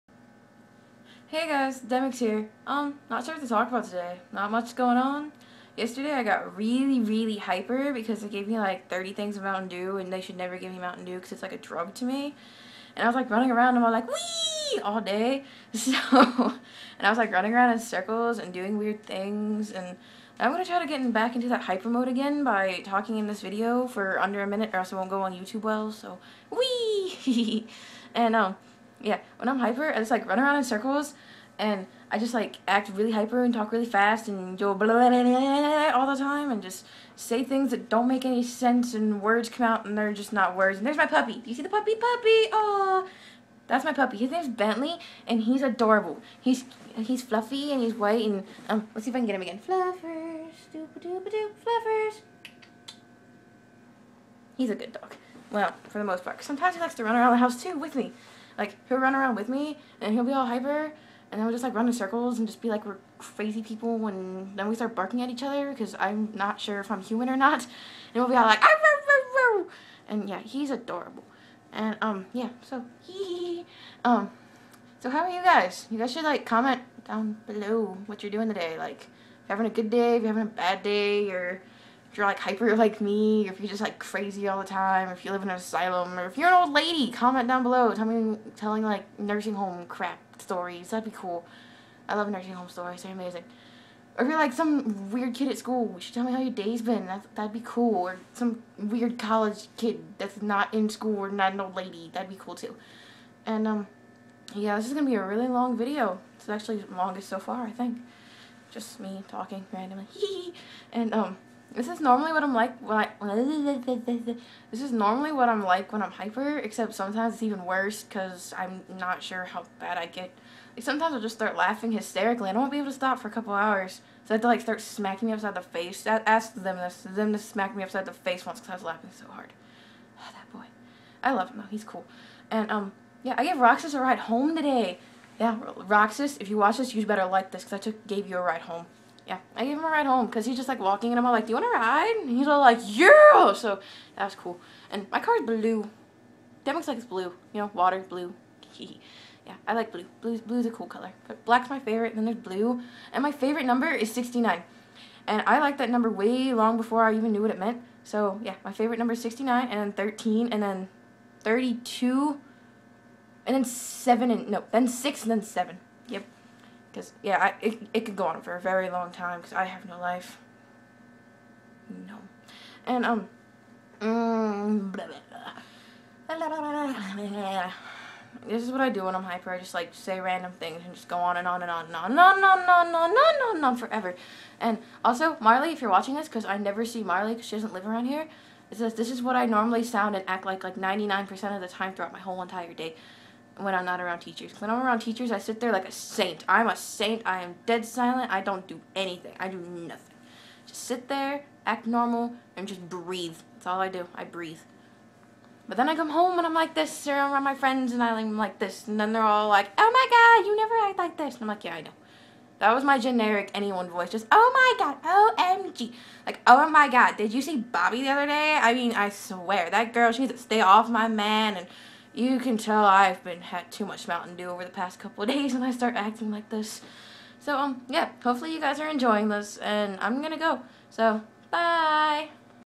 The Daily Life Of Demyx #5
This is normally how I act everyday! Plus I've had too much Mountain Dew! Yay!